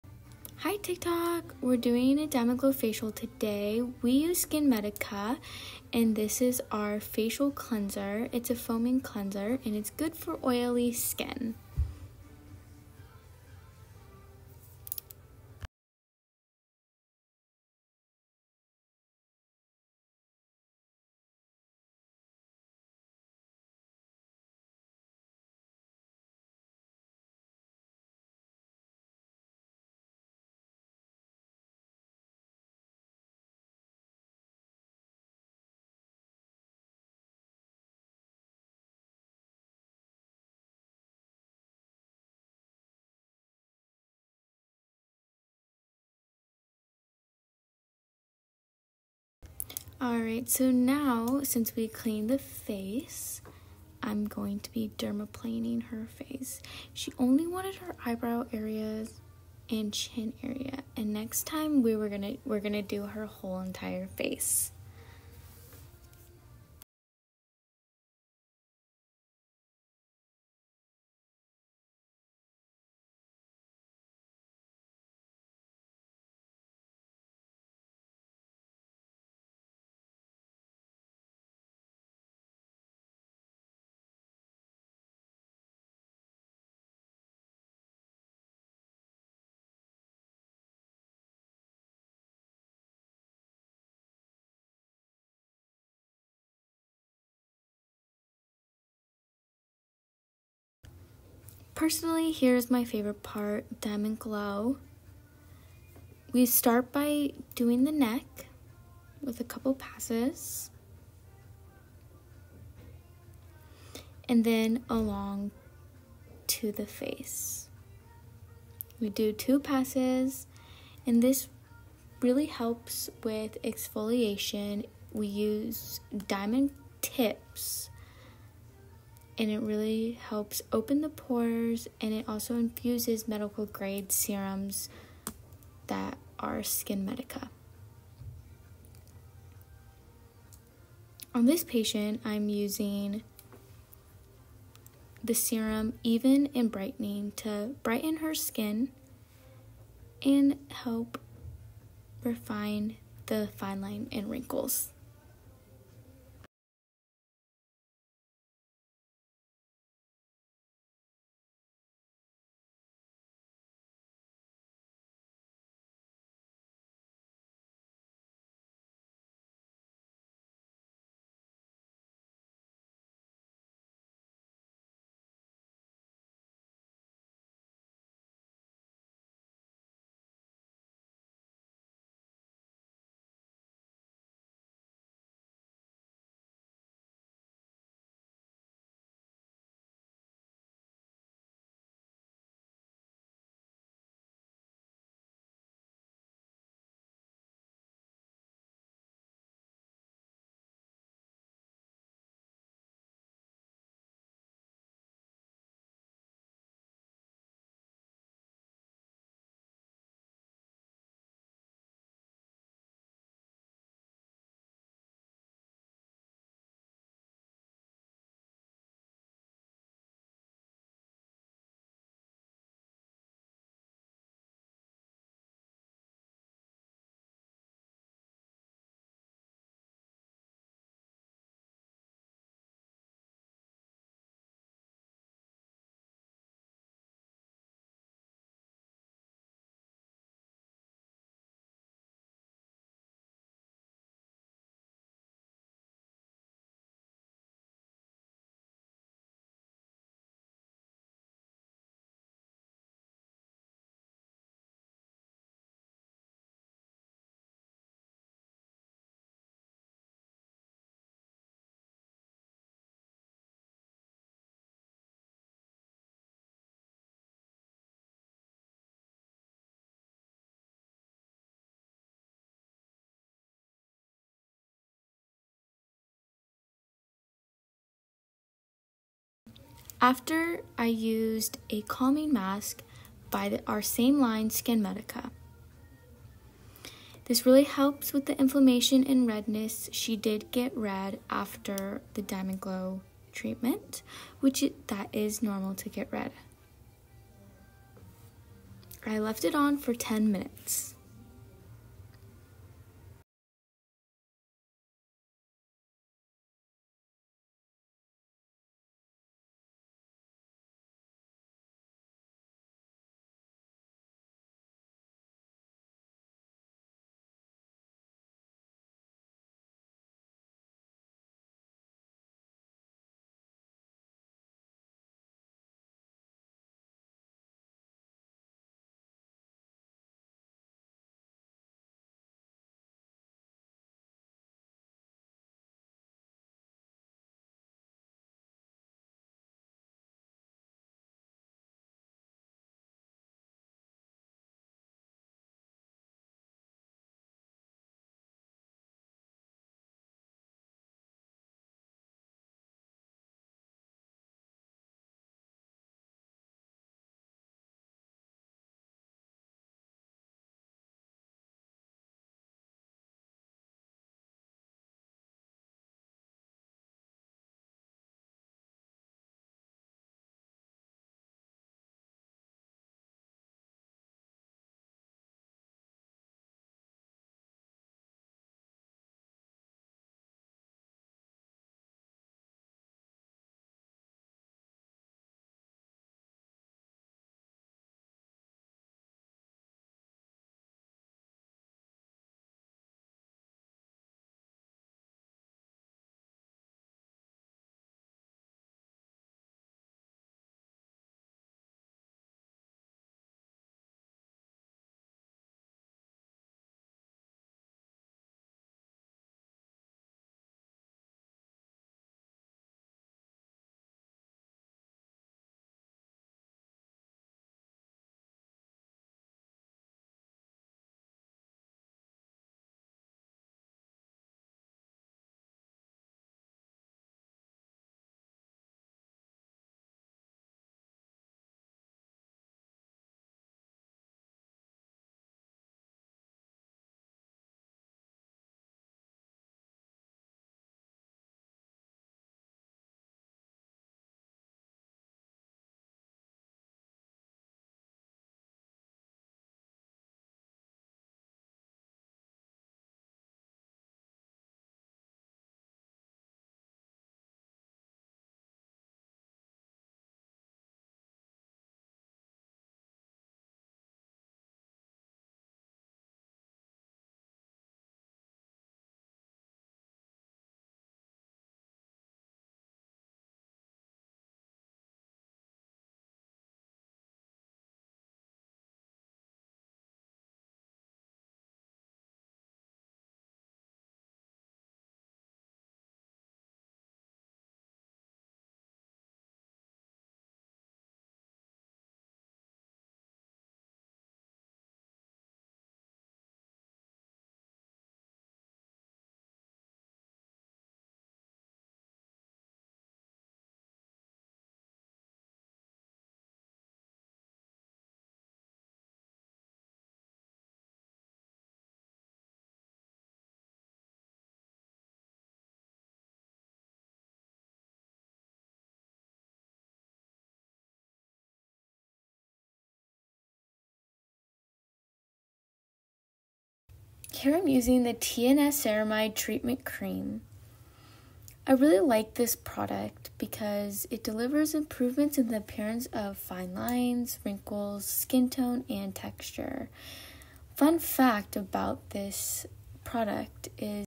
Dermaplaning and Diamondglow💎🩵
💎DiamondGlow is a versatile treatment that will be tailored to suit a variety of skin issues. This treatment is appropriate for people of various skin kinds and ages, and it is adjusted to each patient's exact needs.
Interested in DiamondGlow? Our Danville office is ideally located for clients from San Ramon, Orinda, Blackhawk, Alamo, Livermore, and throughout the Bay Area.